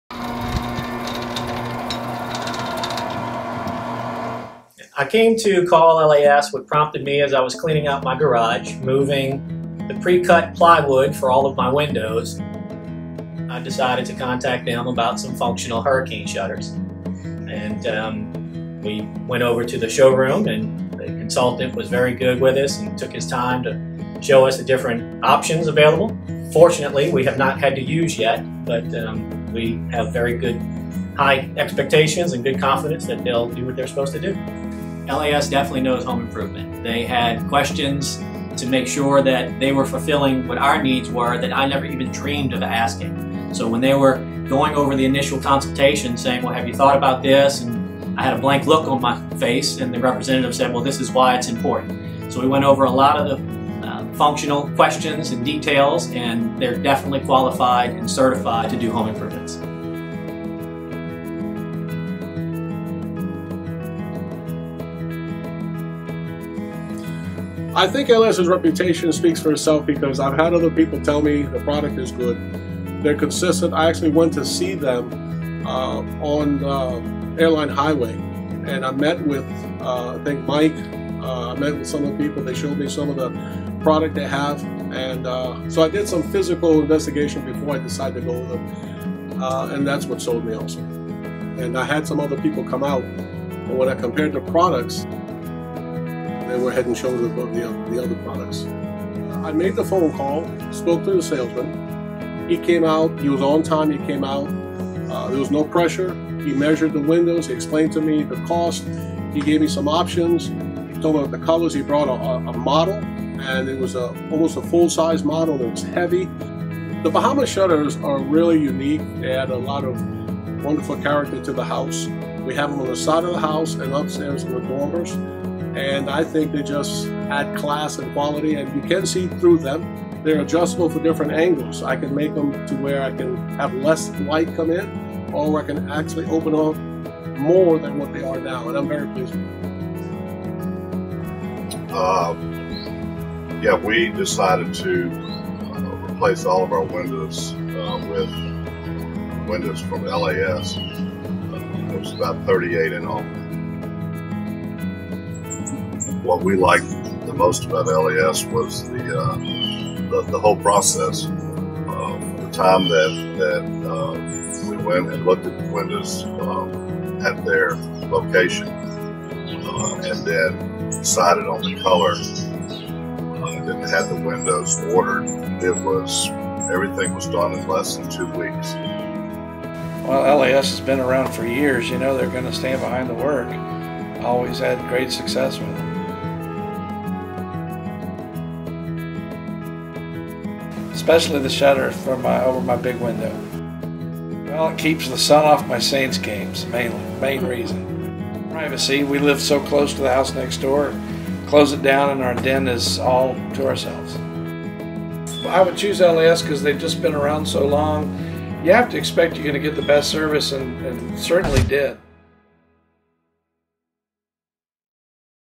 LAS Customer Testimonial 2014
Some of our valued customers sat down with us and talked about how they heard about LAS, why they chose to buy, and how they are enjoying their LAS products.  Watch for customer reviews of LAS HomeGard hurricane shutters, LAS replacement windows and the LAS difference.  A big thank you to all of our customers for choosing to support our locally owned company for the past 50+ years!

For more information about our company, visit us online at LAShome.com